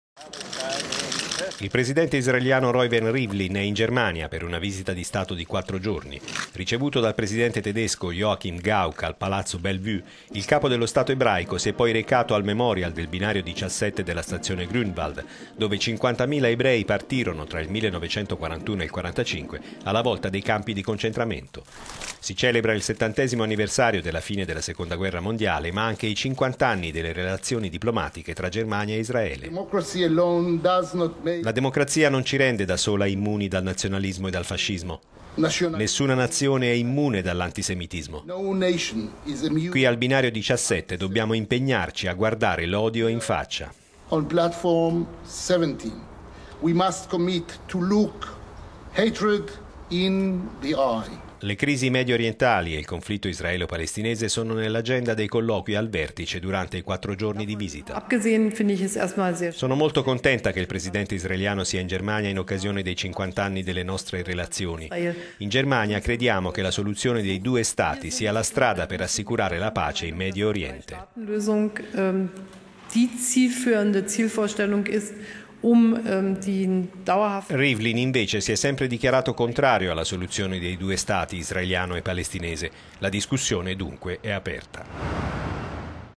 Il presidente israeliano in Germania per 4 giorni di visita dï Stato
Il presidente israeliano Reuven Rivlin è in Germania per una visita di Stato di quattro giorni. Ricevuto dal presidente tedesco Joachim Gauck al palazzo Bellevue, il capo dello Stato ebraico si è poi recato al memorial del binario 17 della stazione Grunewald, dove 50 mila ebrei partirono, trail 1941 e il 45, alla volta dei campi di concentramento. Si celebra il 70.o anniversario della fine della seconda guerra mondiale ma anche i cinquant'anni  delle relazioni diplomatiche tra Germania e Israele…